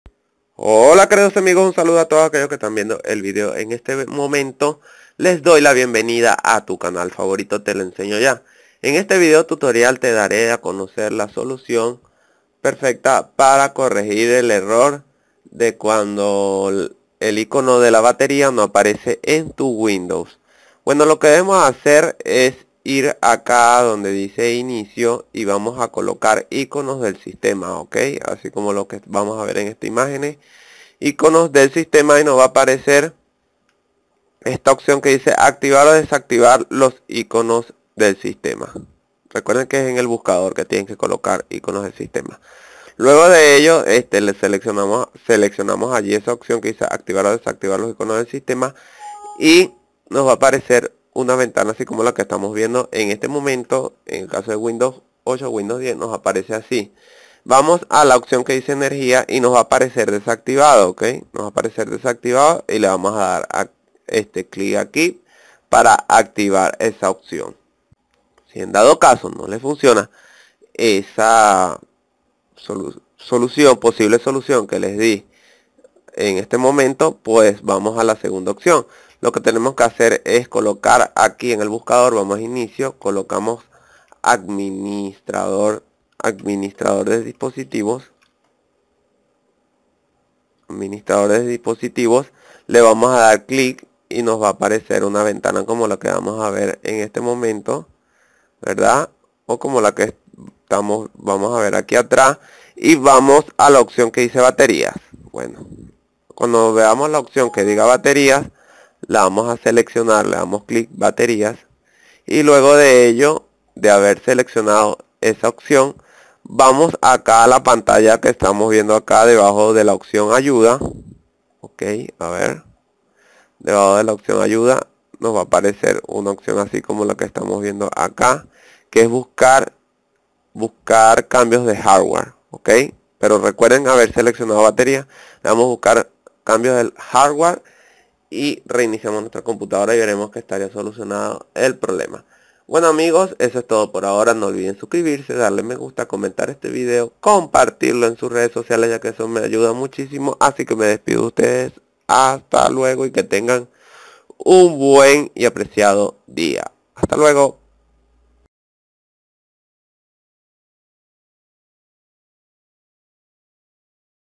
No aparece icono de batería en WIndows I 2 SOLUCIONES 2024
Como activar el icono de bateria en windows 10/8/7 de una manera facil y sencilla / Icono de bateria no aparece?

Videos Recomendados
💛 Como ACELERAR Celular. Haz esto!